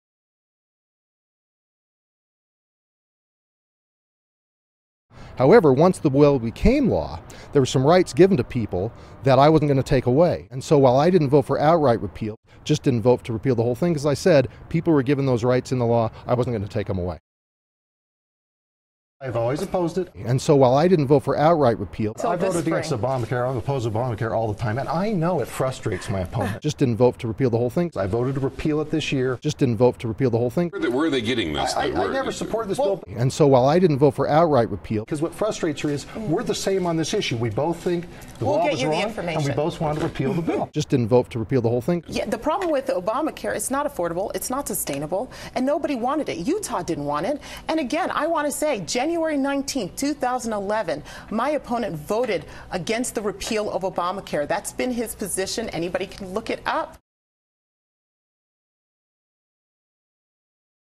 Jim Matheson Doesn't Tell the Truth
Mia Love is running in Utah's new 4th Congressional District. Please support her campaign today.